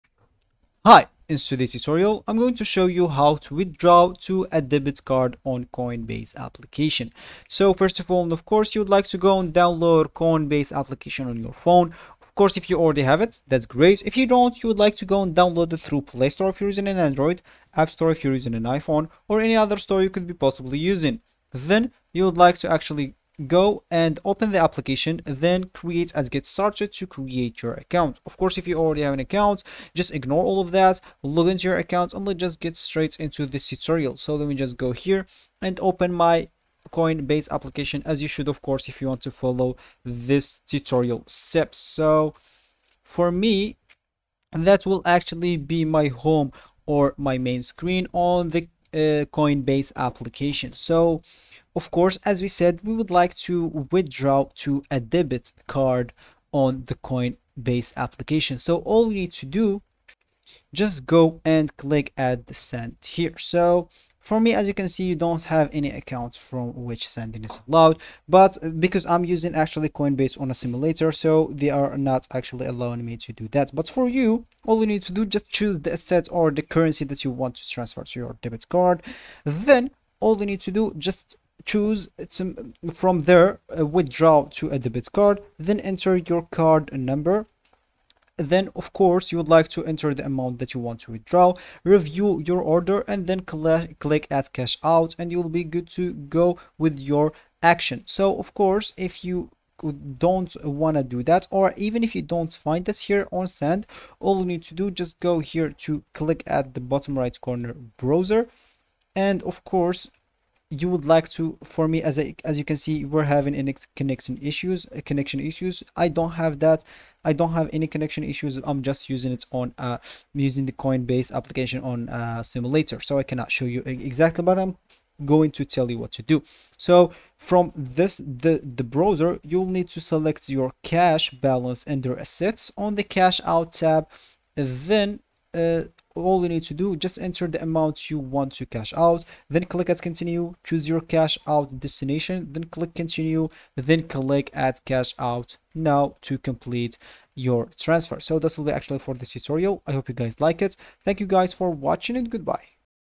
How To Withdraw To Debit Card In Coinbase Tutorial
Today I show how to withdraw to debit card in coinbase tutorial,coinbase tutorial,how to withdraw from coinbase,withdraw money from coinbase